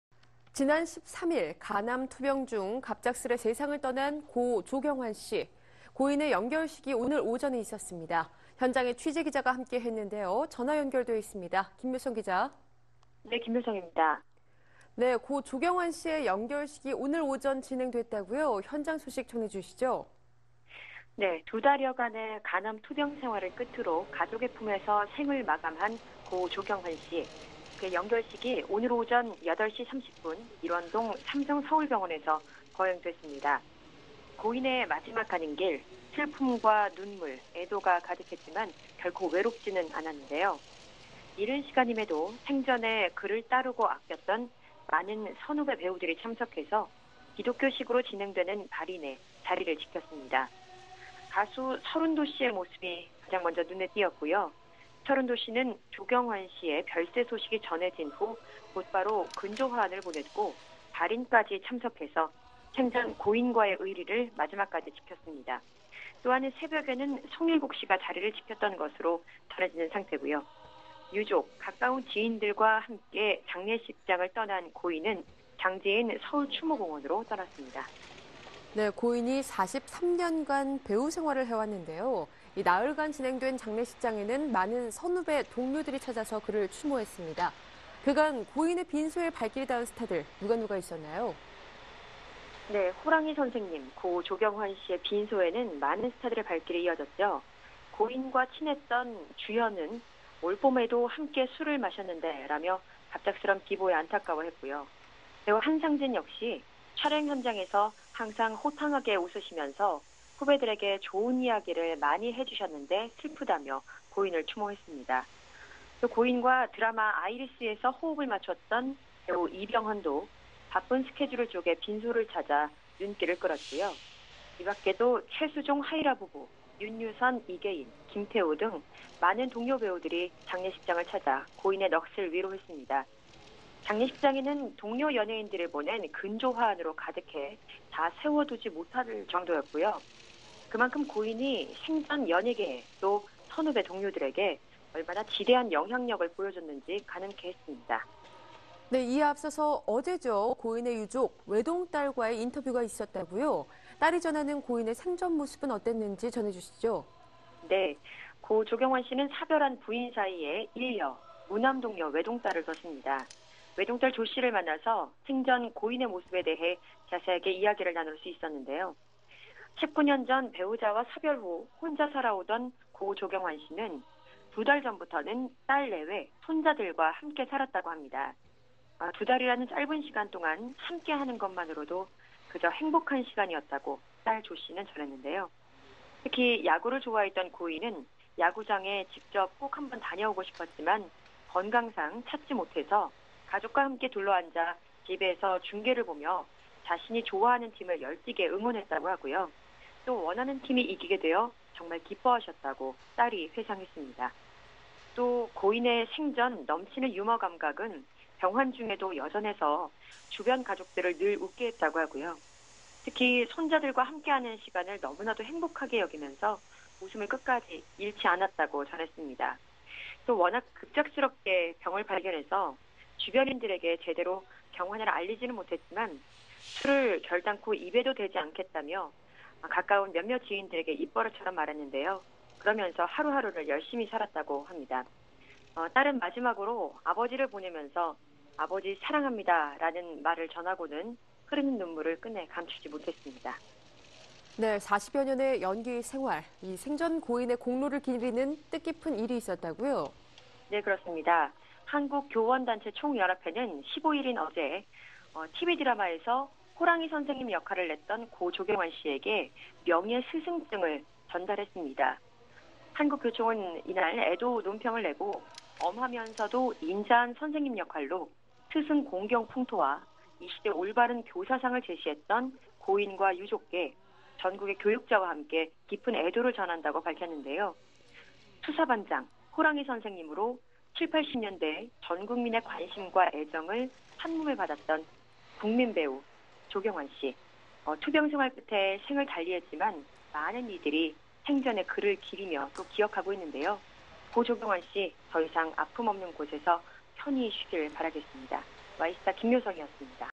[Y-STAR] Cho Kyung-hwan's funeral ('호랑이 선생님' 고 조경환, 영결식 현장)
지난 13일 지병으로 갑작스레 세상을 떠난 고 조경환씨..그의 영결식이 오늘 오전에 있었습니다. 현장에 취재기자가 함께 했는데요 연결해 자세한 소식 전해듣죠. 김묘성 기자

Q) 13일 별세한 고 조경환씨..오늘 오전에는 영결식이 있었다고요 현장 소식 전해주시죠

A) 두 달 여간의 간암 투병생활을 끝으로 가족의 품에서 생을 마감한 고 조경환씨.. 그의 영결식이 오늘 오전 8시 30분 일원동 삼성서울병원에서 거행됐습니다. 고인의 마지막 가는길.. 슬픔과 눈물, 애도가 가득했지만 결코 외롭지 않았는데요. 이른 시간이지만 생전의 그를 따르고 아꼈던 많은 선후배 배우들이 참석해 기독교식으로 진행되는 발인에 자리를 지켰습니다. 가수 설운도씨의 모습이 눈에 띄었고요. 설운도씨는 조경환씨의 별세 소식이 전해진 후 곧바로 근조화환을 보냈고 발인까지 참석해 생전 고인과의 의리를 마지막까지 지켰습니다. 또한 새벽에는 송일국씨가 자리를 지켰던 것으로 전해지고요 유족, 가까운 지인들과 함께 장례식장을 떠난 고인은 장지인 서울 추모공원으로 떠났습니다.

Q) 43년간 배우생활을 해온 고인이기에 나흘간 진행된 장례식장에는 많은 선후배 동료들이 찾아 그를 추모했죠? 그간 고인의 빈소에 발길이 닿은 스타들은 누가누가 있었나요?

A) '호랑이 선생님' 조경환의 빈소에 스타들의 발길이 이어졌습니다. 조경환과 친했던 주현은 "올 봄에도 같이 술을 마셨는데..."라며 갑작스런 비보에 안타까워 했고요.. 한상진 역시 "촬영현장에서 항상 호탕하게 웃으시면서 후배들에게 좋은 이야기를 많이 해주셨는데 슬프다"고 고인을 추모했습니다. 조경환과 드라마 아이리스에서 호흡을 맞췄던 이병헌도 바쁜 스케줄을 쪼개 빈소를 찾아 눈길을 끌었고요 이밖에도 최수종-하희라 부부, 윤유선, 이계인 김태우 등 많은 동료 배우들이 장례식장을 찾아 고인의 넋을 위로했습니다. 장례식장에는 동료 연예인들이 보낸 근조화환으로 가득해 다 세워두지 못하는 정도였고요. 그만큼 고인이 생전 연예계에..또 선후배 동료들에게 얼마나 지대한 영향력을 보여줬는지 가늠케 했습니다.

Q) 네 이에 앞선 어제는 고인의 유족.. 외동딸과의 인터뷰가 있었다구요. 딸이 전하는 고인의 생전모습은 어땠나요?

A) 고 조경환씨는 사별한 부인 사이에 1녀..무남독녀 외동딸을 뒀습니다. 외동딸 조씨를 만나 생전 고인의 모습에 대해 이야기를 나눌 수 있었는데요. 19년 전 배우자와 사별 후 혼자 살아오던 고 조경환씨는 두 달 전부터는 딸내외. 손자들과 함께 살았다고 합니다. 두 달이라는 짧은 시간 동안 함께 하는 것만으로도 행복한 시간이었다고 딸 조씨는 전했는데요. 특히 야구를 좋아했던 고인은 야구장에 꼭 다녀오고 싶었지만 건강상 직접 찾지 못해 가족과 둘러앉아 집에서 중계를 보며 자신이 좋아하는 팀을 열띠게 응원했다고 하고요 또 원하는 팀이 이기게 되어 정말 기뻐하셨다고 딸이 회상했습니다. 또 고인의 생전 넘치는 유머감각은 병환 중에도 여전해 주변 가족들을 늘 웃게 했고요 특히 손자들과 함께 하는 시간을 너무나 행복하게 여기며 웃음을 잃지 않았다고 전했습니다. 워낙 급작스럽게 병을 발견해 주변인들에게 병환을 제대로 알리지는 못했지만 술을 입에도 대지 않겠다고 가까운 몇몇 지인들에게 입버릇처럼 말하며 하루하루를 열심히 살았다고 합니다. 딸은 마지막으로 아버지를 보내며 '아버지 사랑합니다'..라는 말을 전하고는 흐르는 눈물을 감추지 못했습니다.

Q) 한편 생전 고인의 공로를 기리는 뜻깊은 일도 있었다고요?

A) 한국교원단체총연합회는 15일 TV 드라마에서 [호랑이 선생님] 역할을 했던 고 조경환씨에게 명예 스승증을 전달했습니다. 한국교총은 이날 애도 논평을 내고 "엄하면서도 인자한 선생님 역할로 스승공경 풍토와 이 시대 올바른 교사상을 제시했던 고인과 유족께 전국의 교육자와 함께 깊은 애도를 전한다"고 밝혔는데요. 수사반장, 호랑이 선생님으로 70, 80년대 전 국민의 관심과 애정을 한 몸에 받은 국민 탤런트 조경환씨. 투병생활 끝에 생을 달리했지만 많은 이들이 생전의 그를 기리며 기억하고 있는데요. 고 조경환씨..더 이상 아픔 없는 곳에서 편히 쉬길 바랍니다. Y-STAR 김묘성이었습니다.